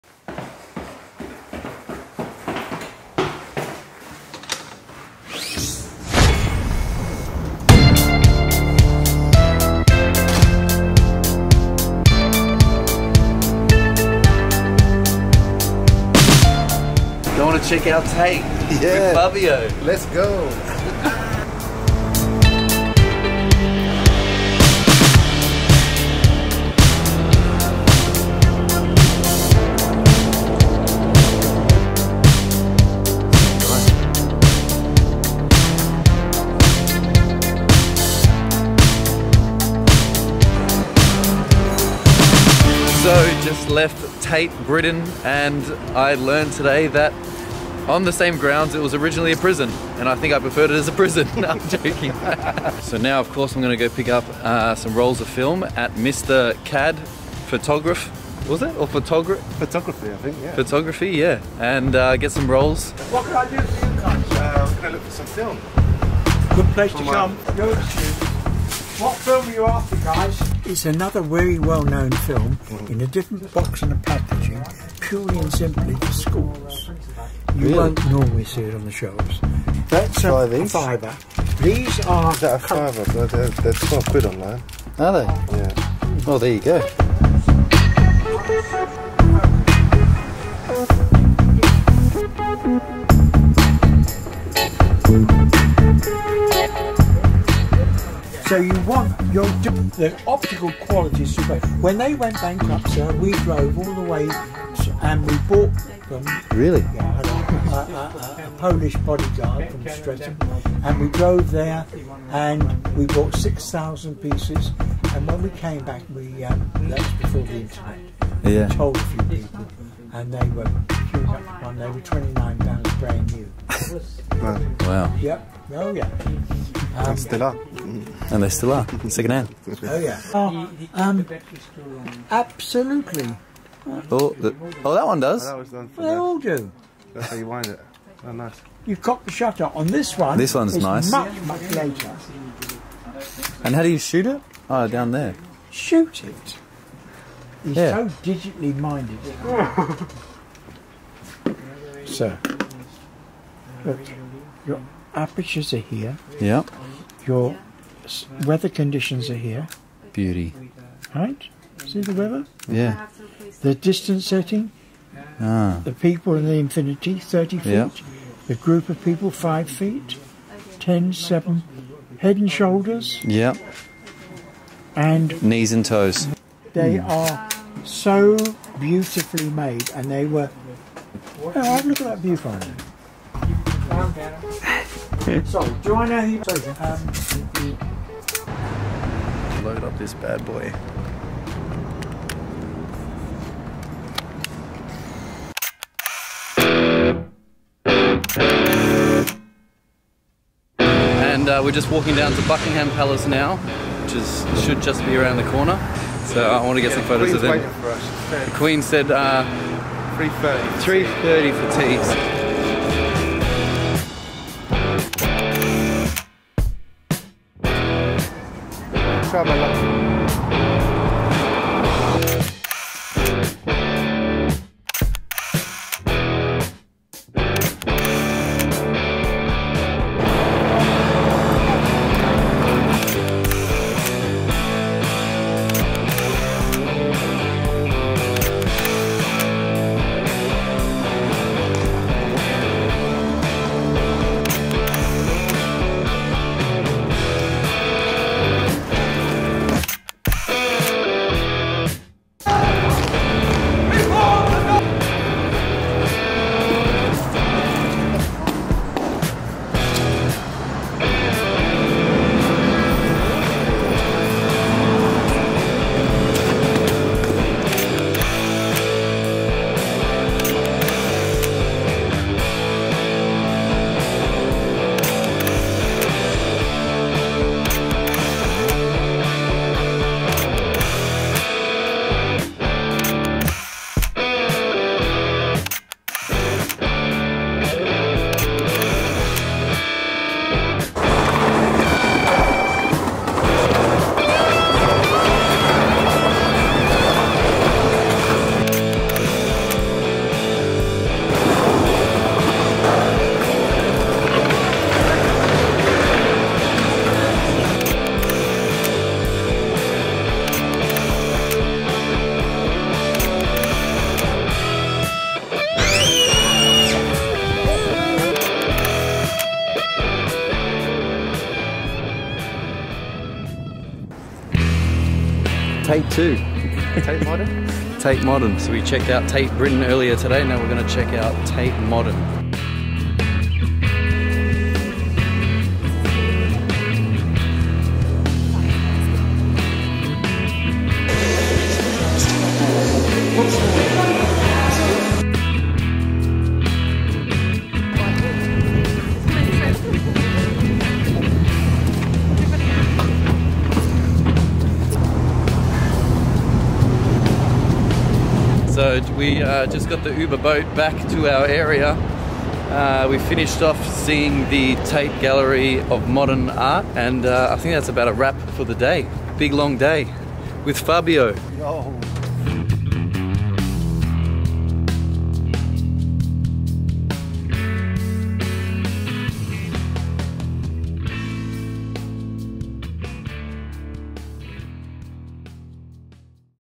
London Street Photography | London Vlog [Kodak ColorPlus 200]
Video Chapters
FRIDAY
0:00  - Intro
0:23- Tate Britain
1:04 - Mr. CAD Photographic - Film Pickup
3:15 - Street Film Photography
5:25 - Tate Modern
5:47 - Outro

Nice day out in London with the grand tour guide Fabio  (@fabiopomaro) Such a great city to shoot some film street photography. Would be nice to have a few more days and a few more rolls of 35mm film.

recently I've been shooting quite a bit of ColorPlus 200 for a couple of reasons, of course, because of the price (it's cheap) but I also like it because of the grain, I'm currently not a fan of fine grain, I want my shots to look like 35mm film shots and I feel I can achieve this with the Kodak ColorPlus 200... Any that's just my 2 cents on the subject.

All shots were taken on the Klasse S FujiFilm point and shoot.

📸 Equipment:
- Fujifilm Klasse S
- 1 x Rolls - Kodak Colour Plus 200 [35mm]
- Video on the Sony A7iii